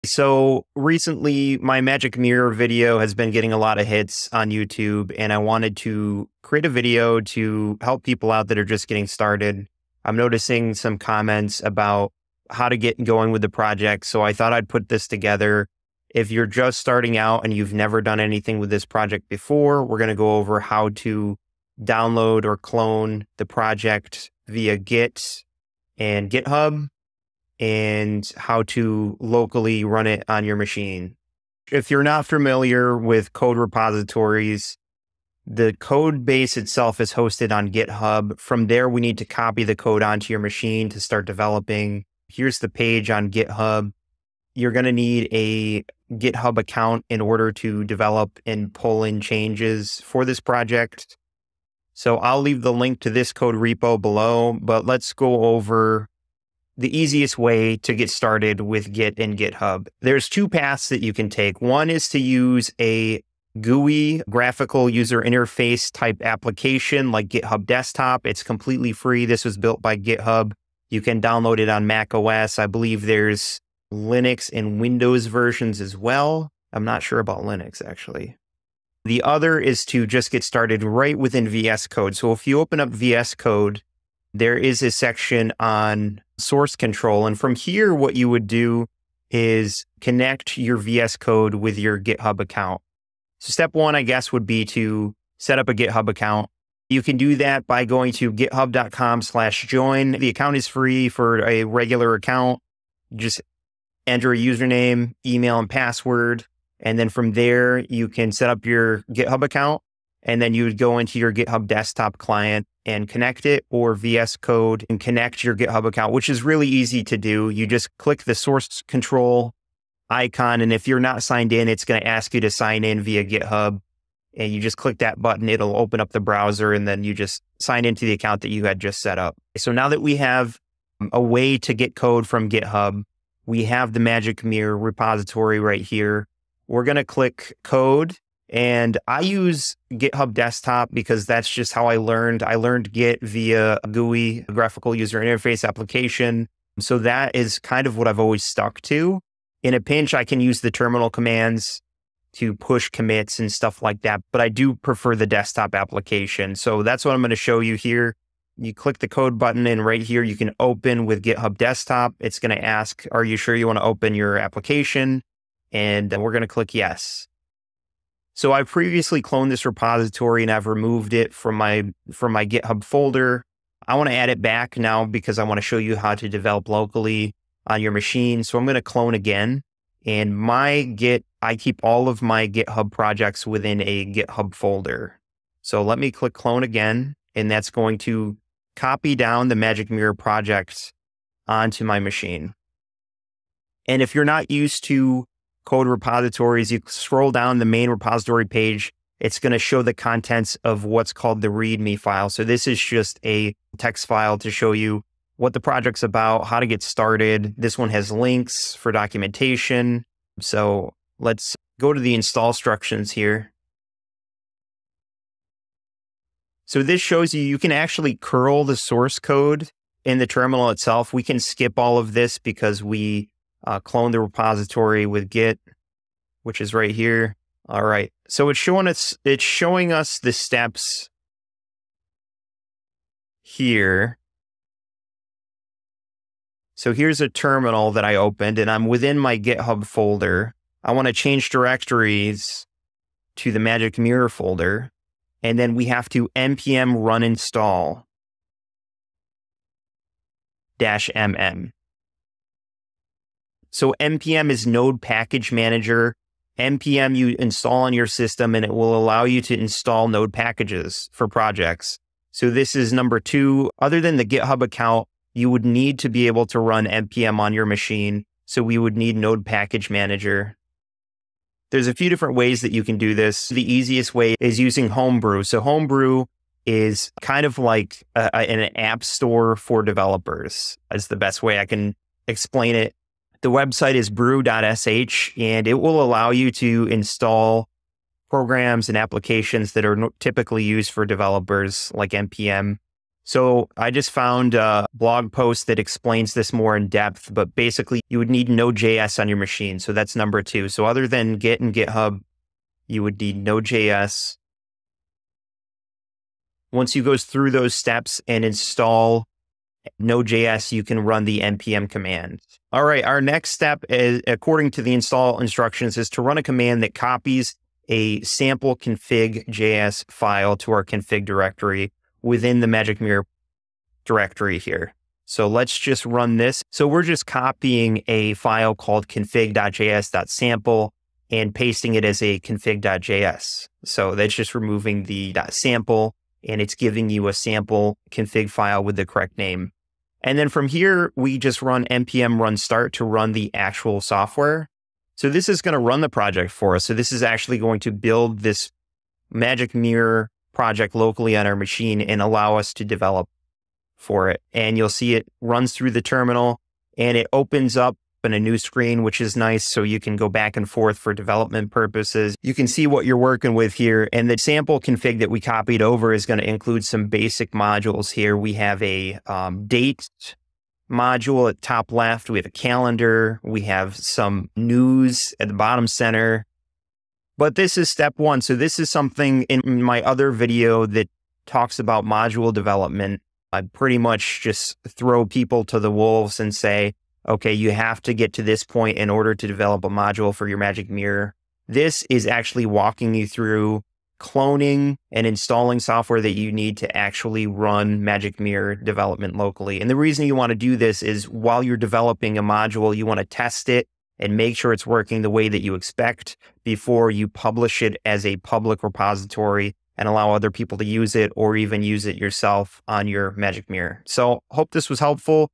Magic Mirror - Getting Started with Local Development
This shows the first steps towards developing for the Magic Mirror community project. This includes setting up a github account, installing Node, cloning the Magic Mirror repo, and running it locally.